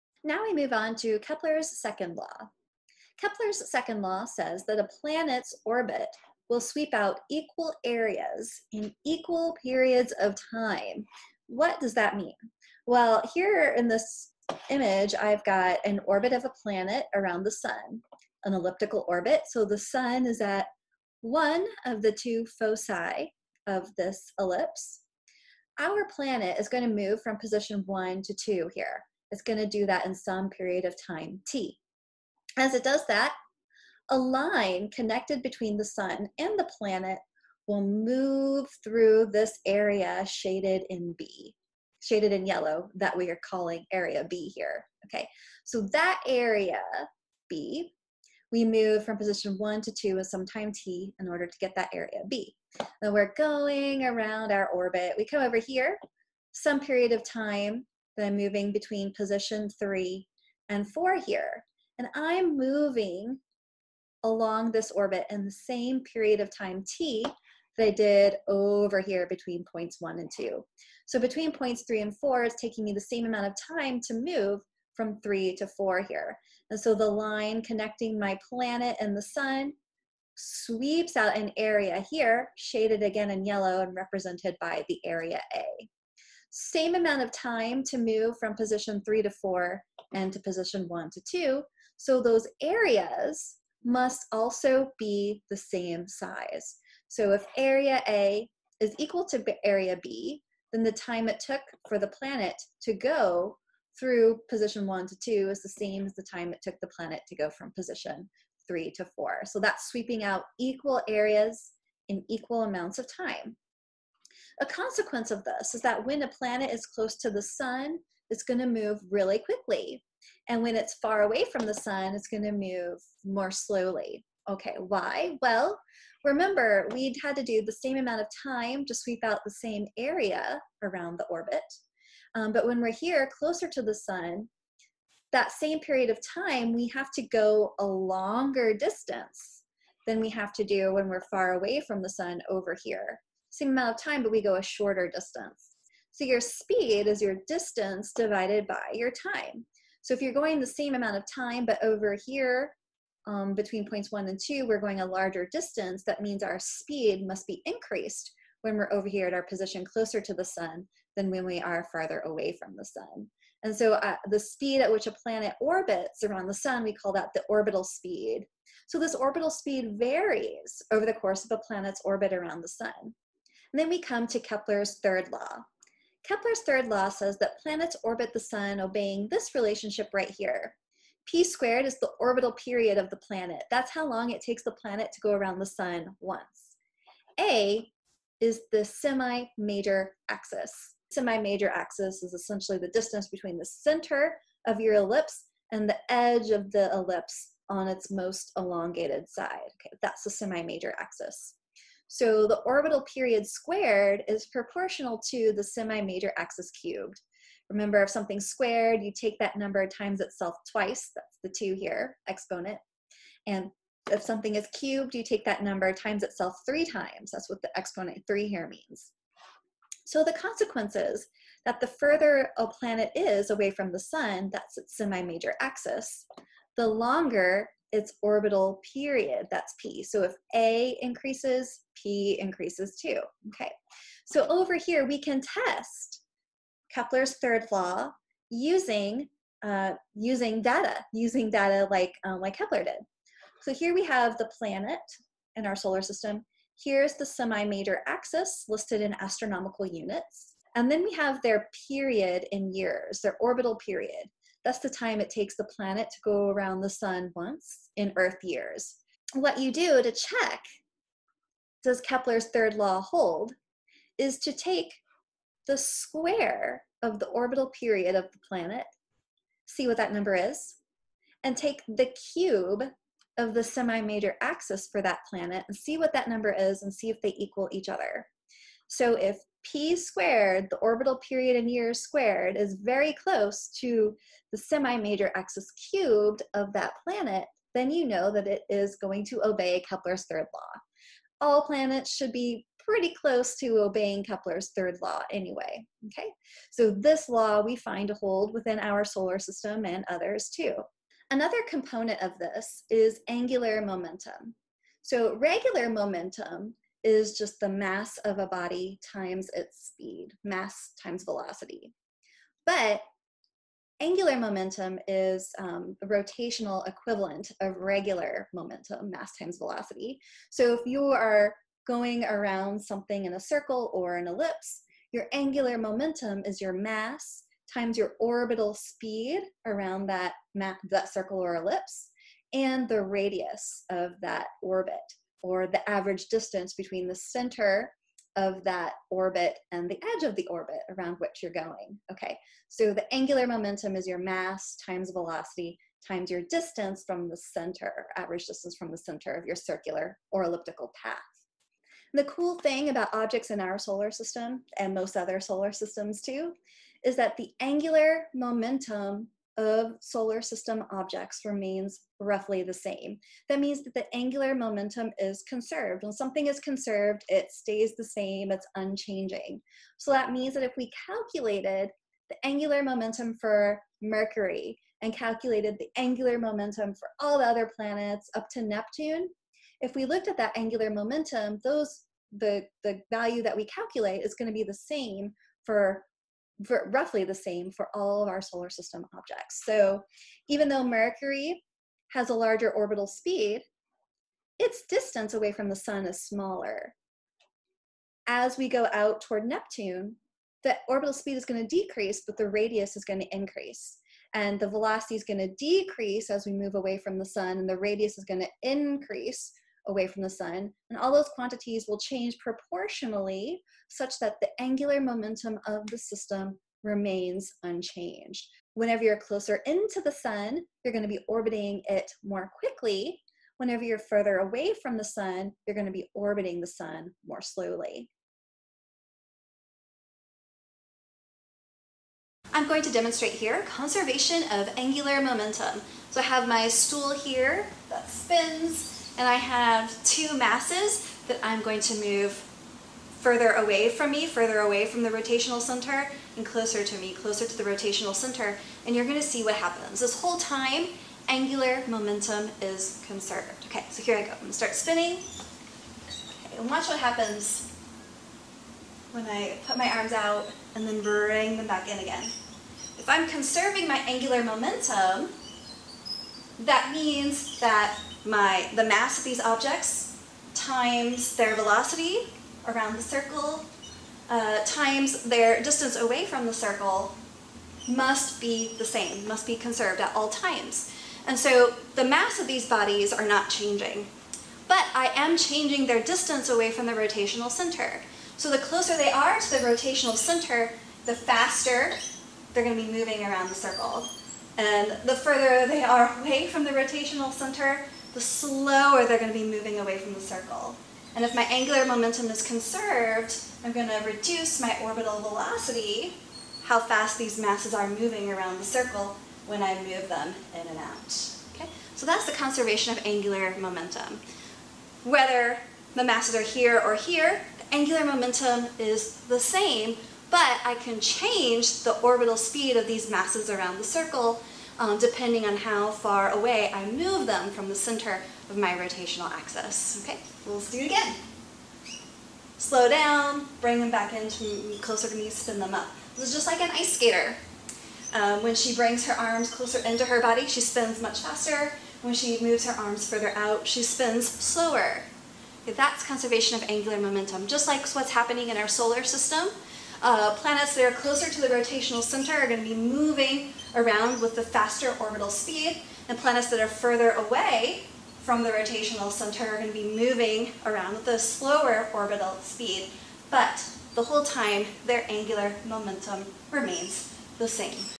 Week 5 PHY 105 - Kepler's Second and Third Laws
In this mini-lecture, we explore Kepler's Second and Third Laws of Planetary Motion. This video is for Dr. Maria Weber's PHY 105 - Astronomy course at Delta State University.